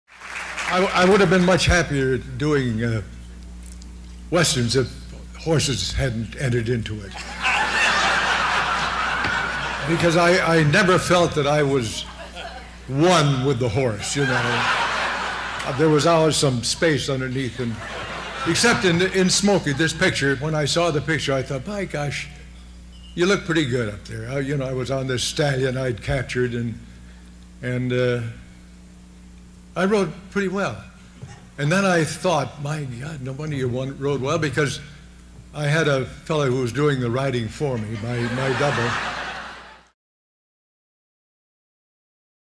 Fred MacMurray: The Golden Boot Awards
Fred MacMurray at the 4th Golden Boot Awards in 1986.